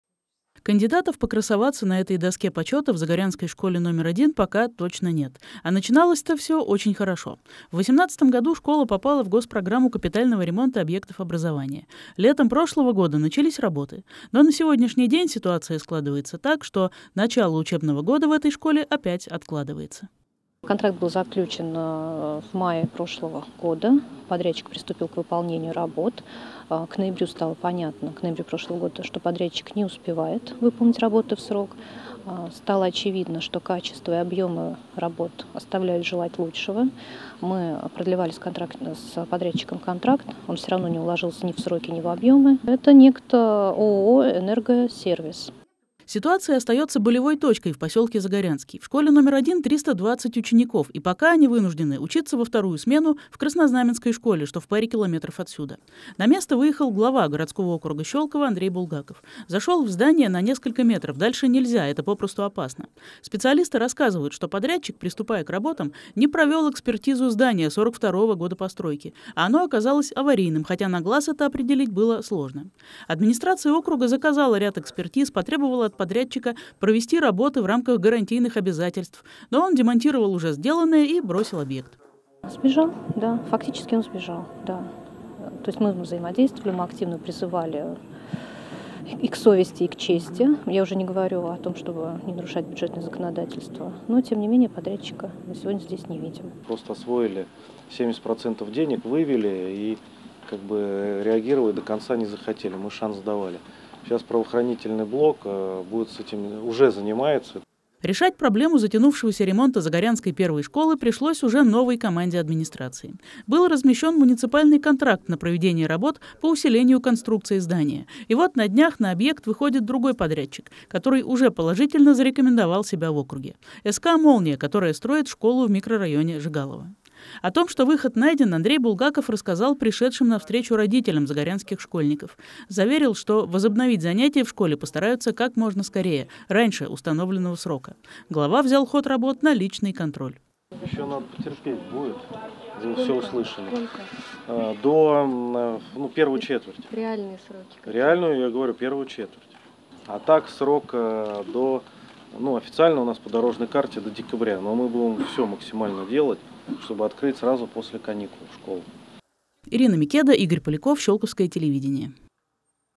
В Загорянской школе №1 – новый подрядчик
Возобновляются ремонтные работы в школе №1 посёлка Загорянский. Проблемный объект, который закрыт уже больше года, берётся закончить новый подрядчик, СК-Молния. Рабочее совещание на объекте провёл глава городского округа Андрей Булгаков.